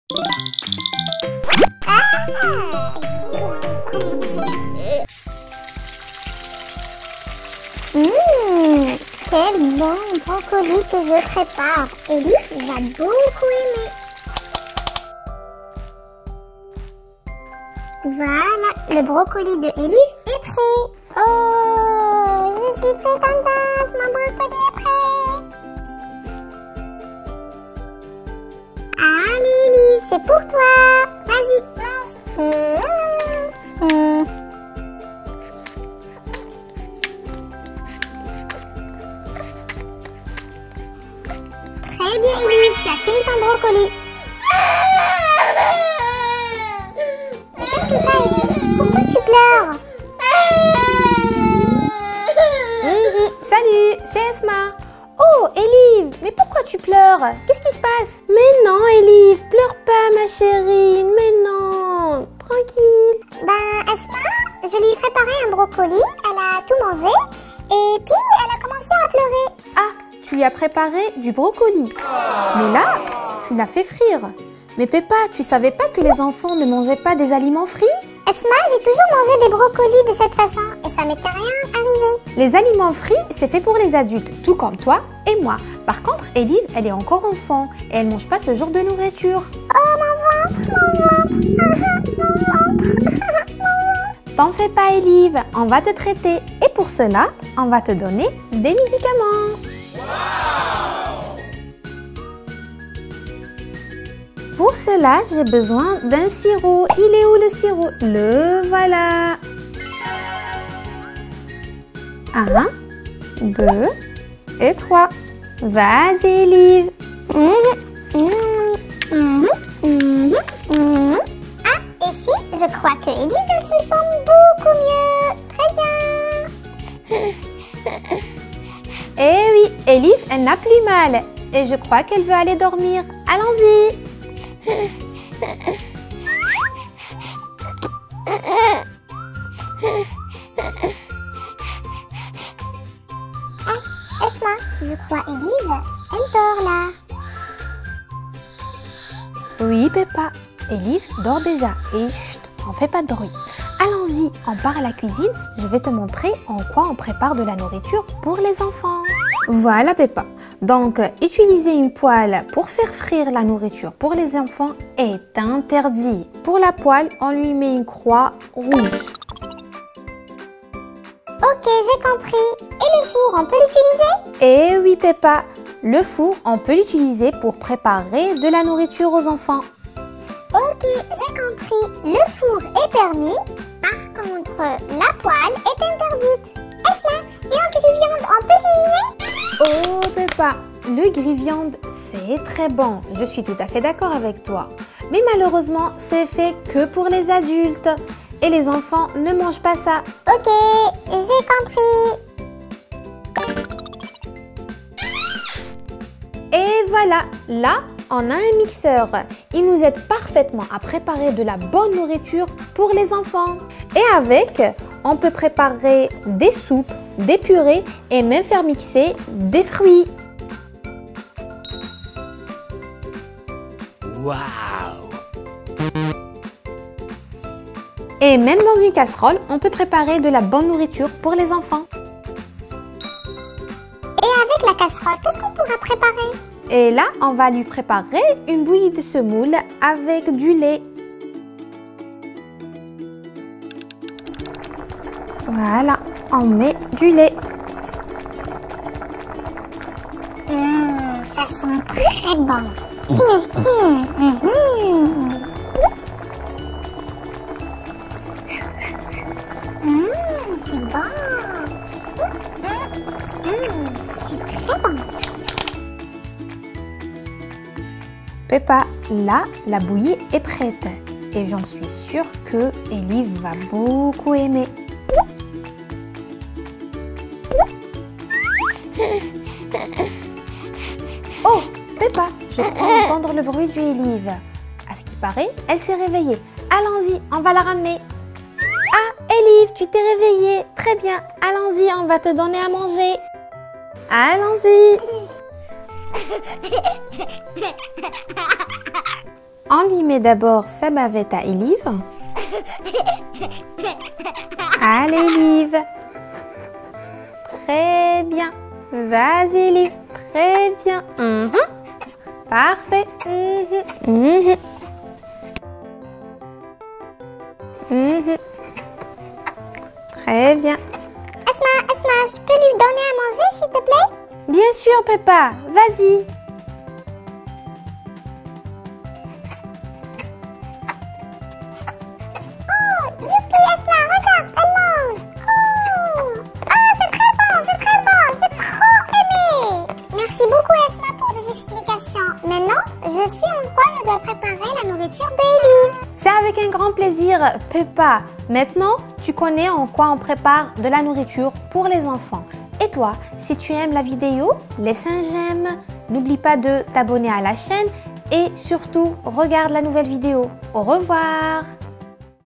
Cuisinons ensemble pour Baby Alive ! 👶 Jeux comme maman pour enfants.
Voici nos jeux comme maman pour enfants avec Baby Alive et Peppa Pig. Apprenons à cuisiner pour les enfants. Préparons à manger ensemble pour Baby Alive.

À bientôt !